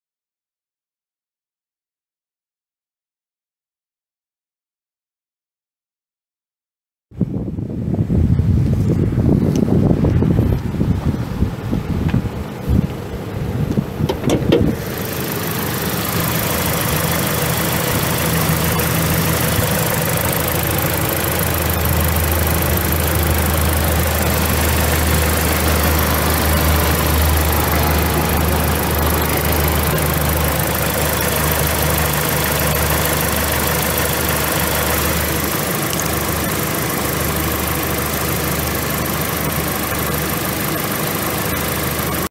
2009 Chevrolet Aveo5 for sale | no-reserve Internet auction October 25, 2017
2009 Chevrolet Aveo5
217,201 miles on odometer
1.6L L4 DOHC 16V gas engine
Automatic transmission
AC and heat
AM/FM
Manual windows and locks
Cloth interior
185/60R14 tires
AC does not blow cold
Missouri title
KL1TD66E99B311569
It's located in Boonville, MO, and we're selling it to the highest bidder regardless of price.